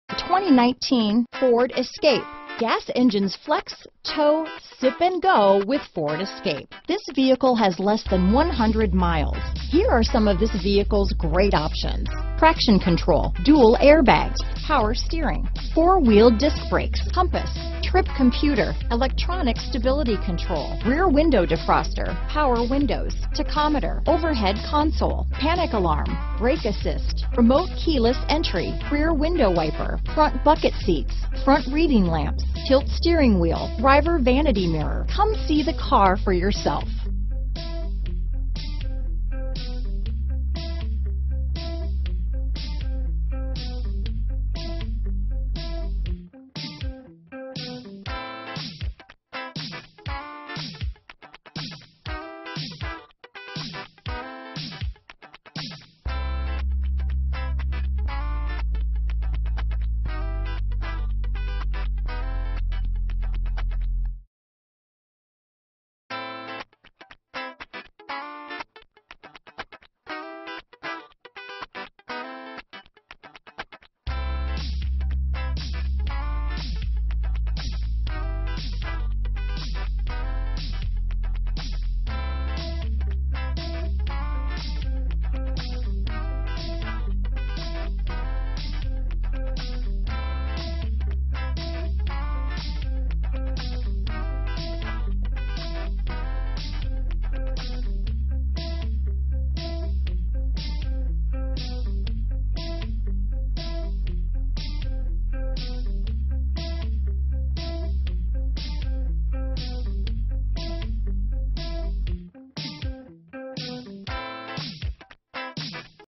2019 Ford Escape Statesboro, Metter, Augusta, Dublin, Claxton, GA SEE9037
Black N 2019 Ford Escape available in Statesboro, Georgia at J.C. Lewis Ford Statesboro. Servicing the Metter, Augusta, Dublin, Claxton, GA area.

Black Metallic 2019 Ford Escape S FWD 6-Speed Automatic 2.5L iVCTbrbrRecent Arrival! 21/29 City/Highway MPG Price does not include Tax, Title and License fees; Price does include: $1,000 - Retail Bonus Customer Cash. Exp. 07/01/2019, $3,500 - Retail Customer Cash. Exp. 06/02/2019